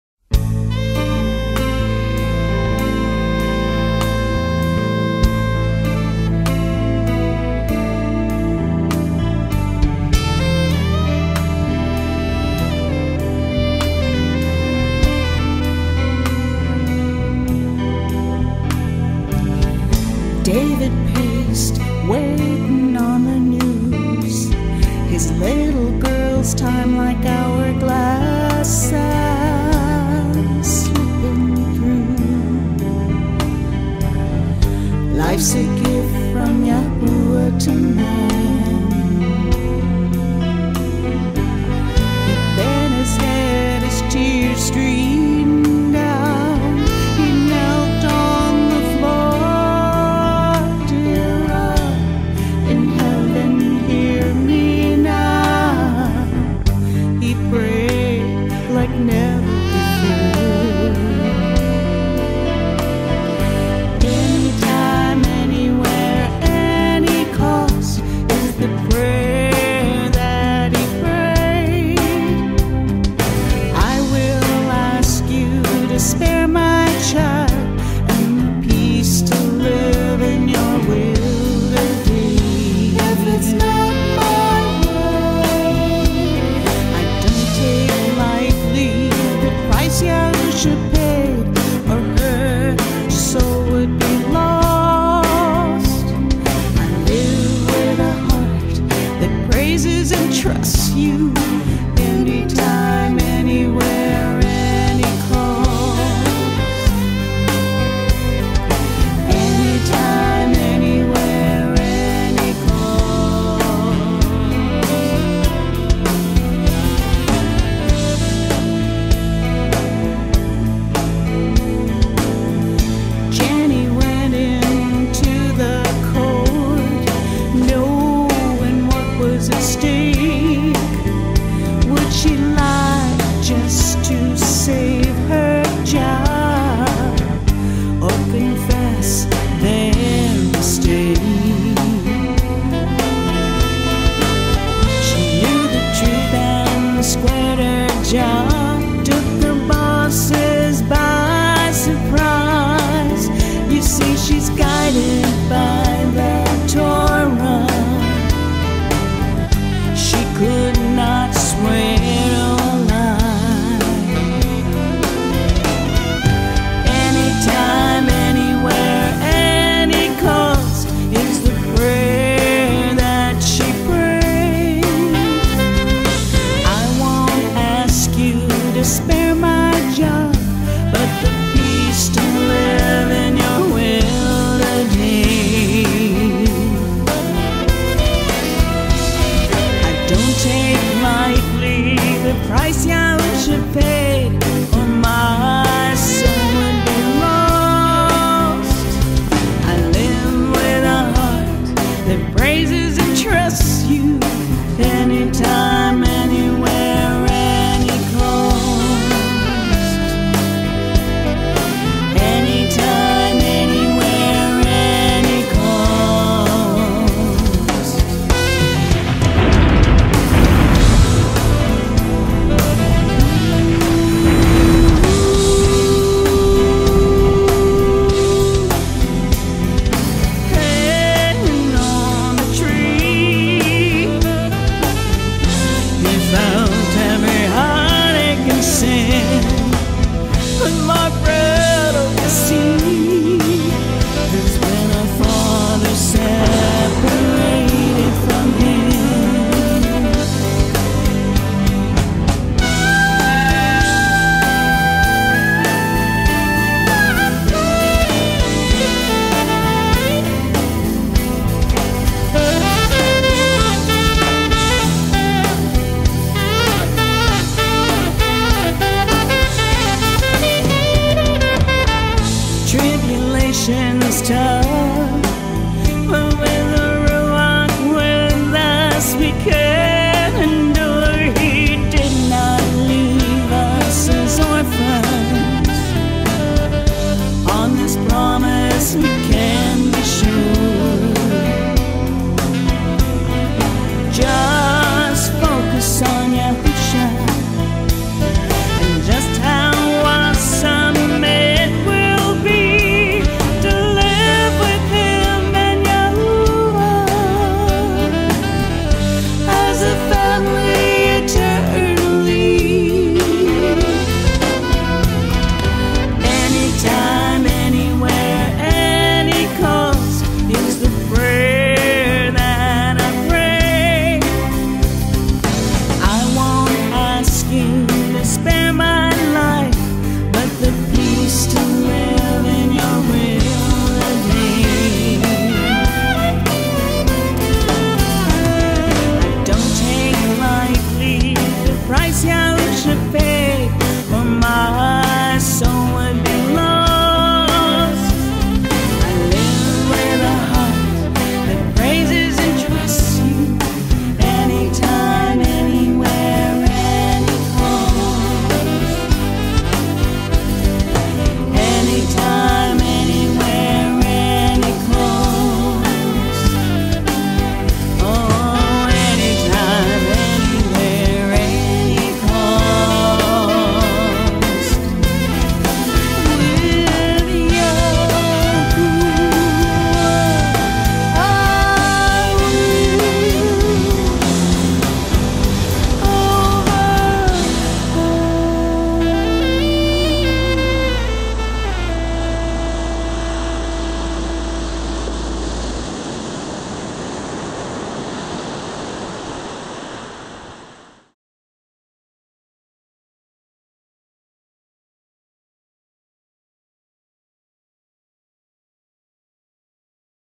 Anytime Anywhere Any Cost HD
In times of tribulation we are told to endure, and with Yahuah and Yahusha we can.  What a blessing to be in this time knowing the Father and His Son, who gave us a way to come back.  Know that you are loved and blessed and can Trust our Ab.